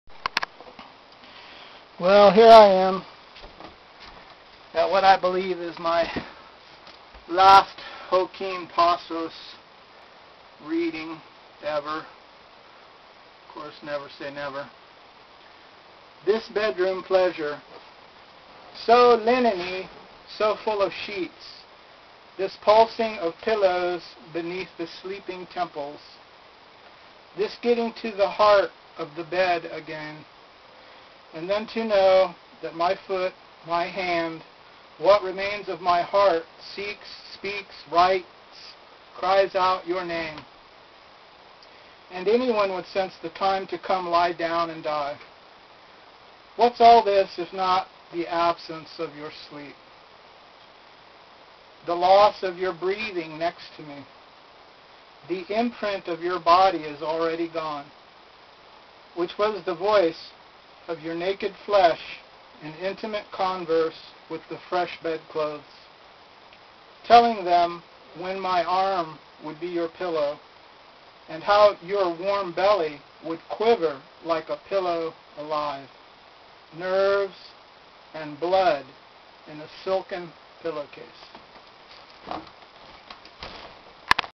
final pasos readng
When in Nicaragua last.. I found this guys house... so looked up some of his poetry.. liked a lot of it so thought to experiment with poetry reading.. and pay him a tribute at the same time.